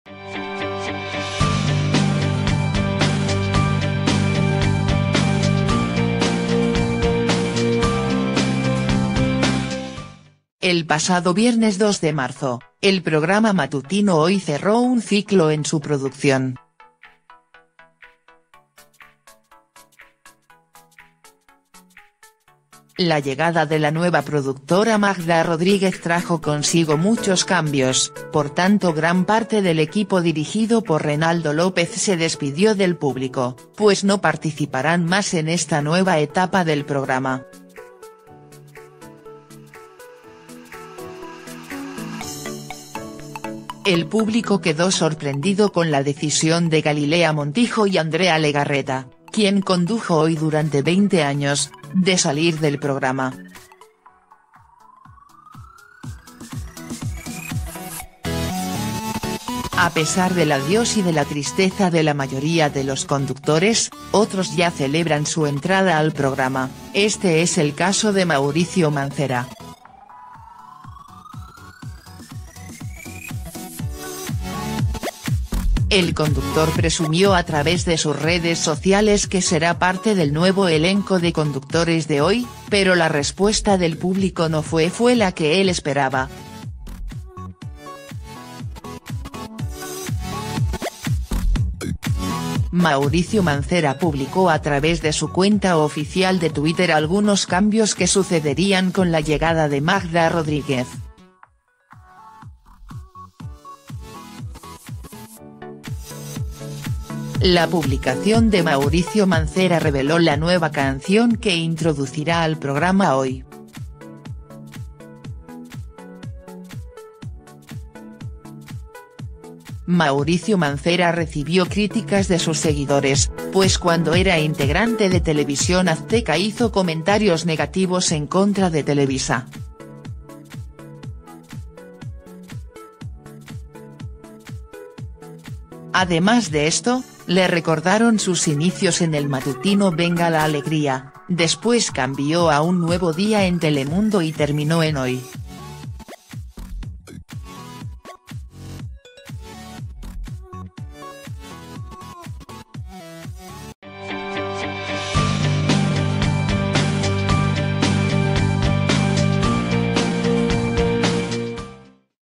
Redes sociales destrozan a Mauricio Mancera tras llegar a Hoy
El pasado viernes 2 de marzo, el programa matutino Hoy cerró un ciclo en su producción. La llegada de la nueva productora Magda Rodríguez trajo consigo muchos cambios, por tanto gran parte del equipo dirigido por Reynaldo López se despidió del público, pues no participarán más en esta nueva etapa del programa.

El público quedó sorprendido con la decisión de Galilea Montijo y Andrea Legarreta, quien condujo Hoy durante 20 años, de salir del programa. A pesar del ‘adiós’ y de la tristeza de la mayoría de los conductores, otros ya celebran su entrada al programa; este es el caso de Mauricio Mancera.

Fuente: goo.gl/QQYKre